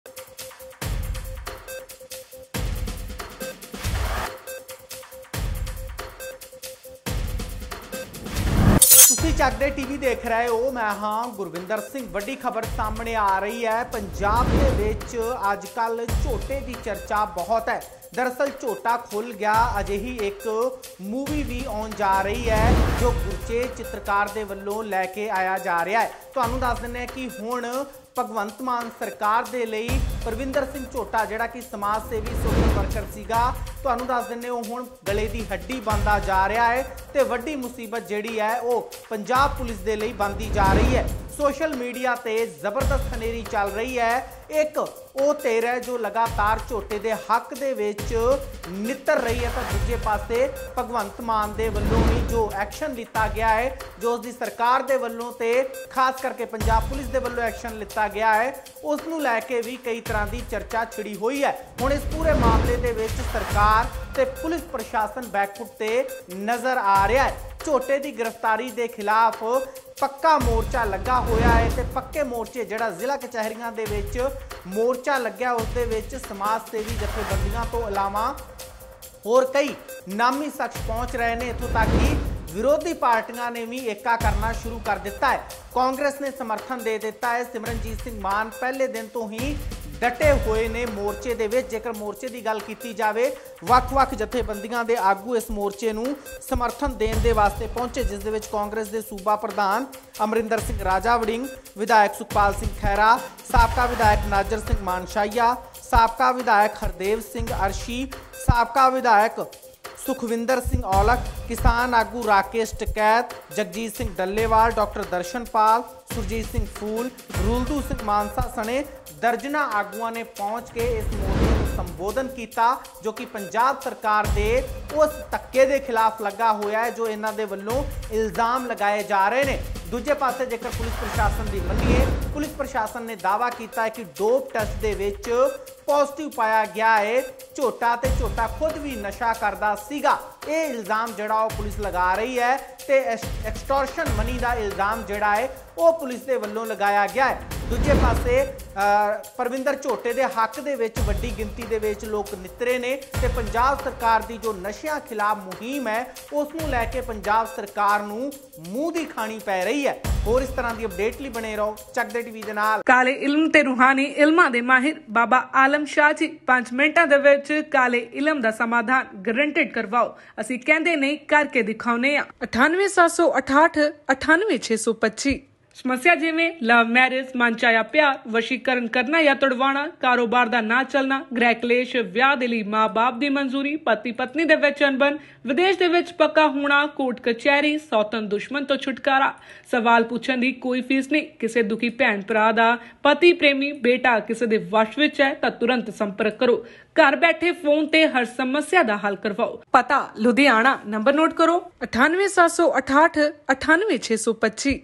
ਖੁੱਲ ਗਿਆ ਝੋਟਾ, ਸੀਐੱਮ ਮਾਨ ਲਈ ਬਣੀ ਮੁਸੀਬਤ ...ਝੋਟੇ ਨੇ ਮਾਨ ਸਣੇ ਸਰਕਾਰ ਦੀ ਕੀਤੀ ਨੀਂਦ ਹਰਾਮ
ਖੁੱਲ ਗਿਆ ਝੋਟਾ, ਸੀਐੱਮ ਮਾਨ ਲਈ ਬਣੀ ਮੁਸੀਬਤ ...
ਝੋਟੇ ਨੇ ਮਾਨ ਸਣੇ ਸਰਕਾਰ ਦੀ ਕੀਤੀ ਨੀਂਦ ਹਰਾਮ